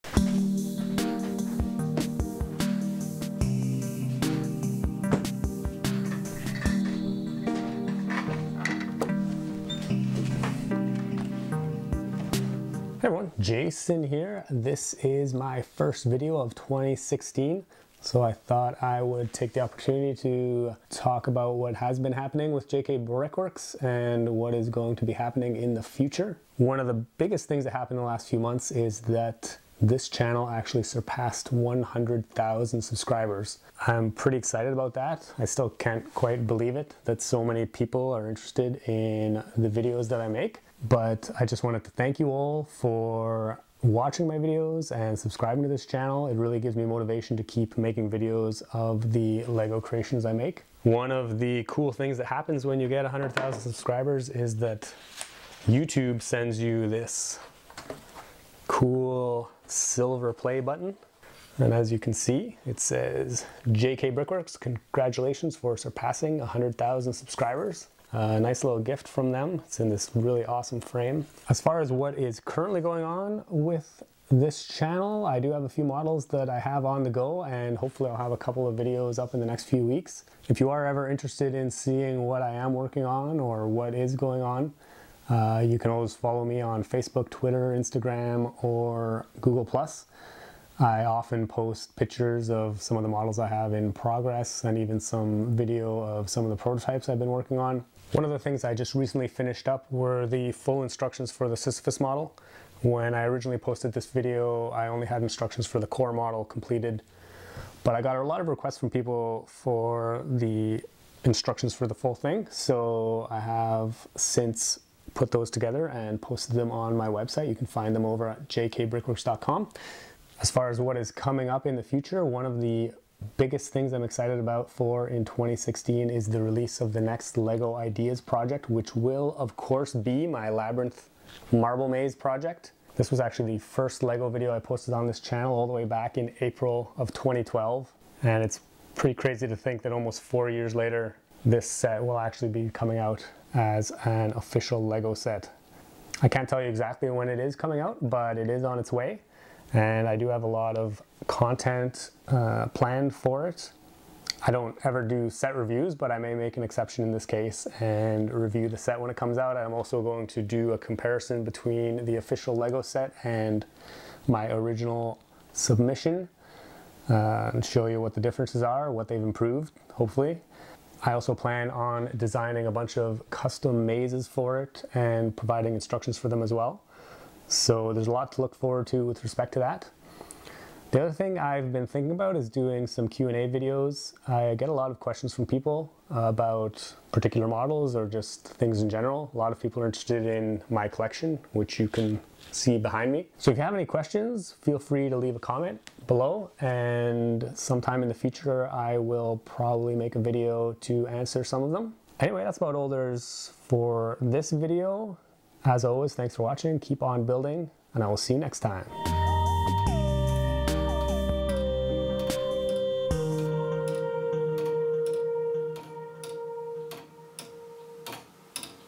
2016 Update - 100,000 subs, LEGO Marble Maze
Just a quick update for 2016, where I talk about passing 100,000 subscribers, what I'm currently working on and plans for the future.

For sneak peeks and behind the scenes content you can follow me on these other sites.

Jason